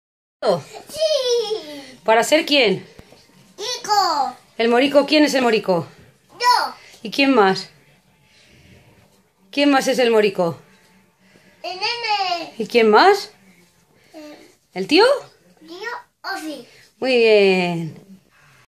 Cabezudo el morico 2016
Ideas de goma eva para los niños barata y rapida careta de cabezudo